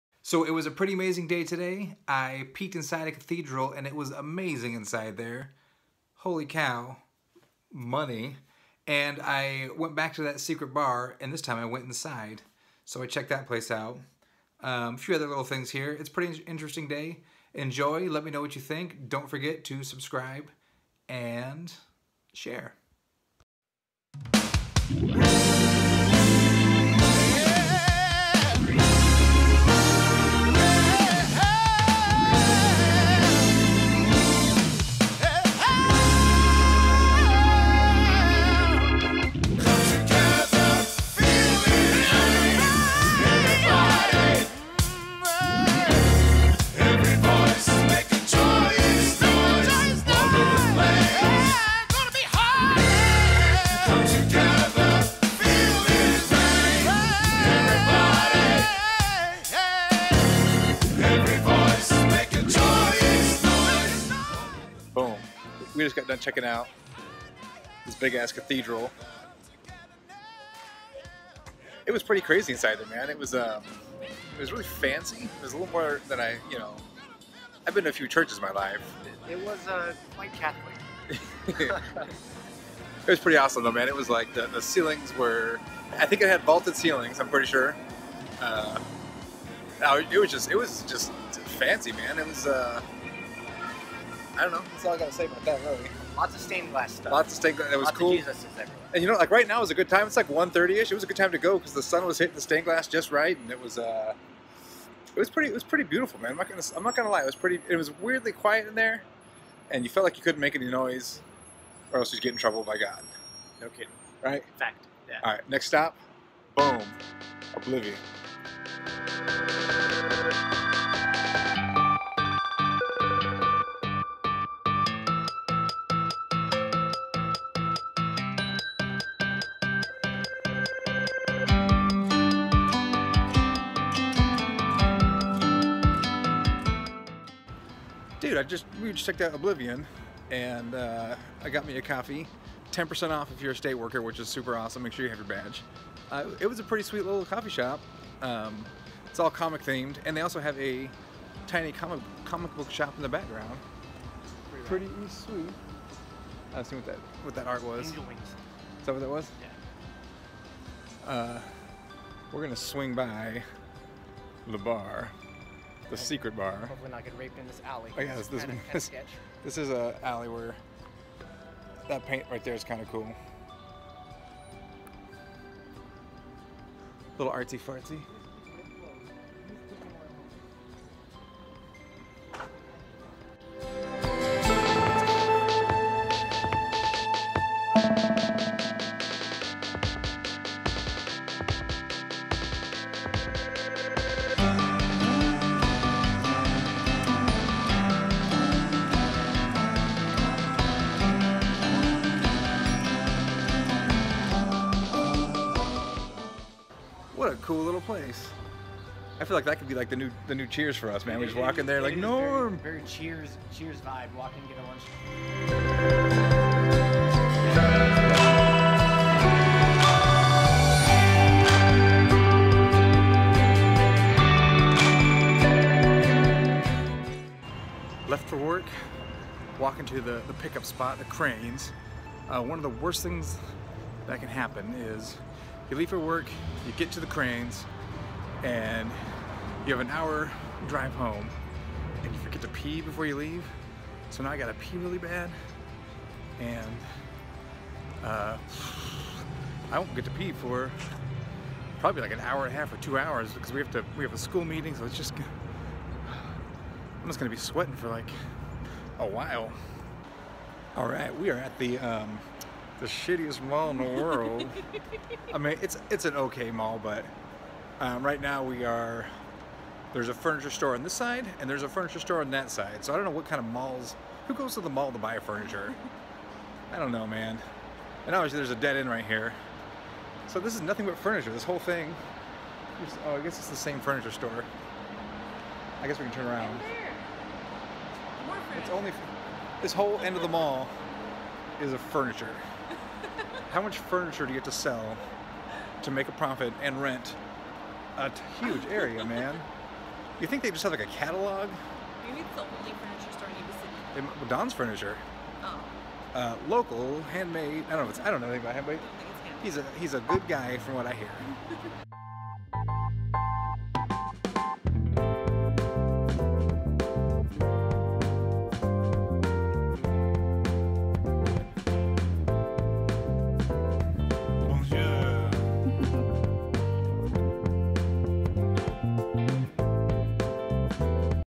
026 - Cathedral and Coffee | Shopping with Shiloh
My buddy and I take a peek at an amazing cathedral, followed by some comic book coffee. Then I finish up the day shopping with Shiloh!

I can always use a little motivation and inspiration from people like you!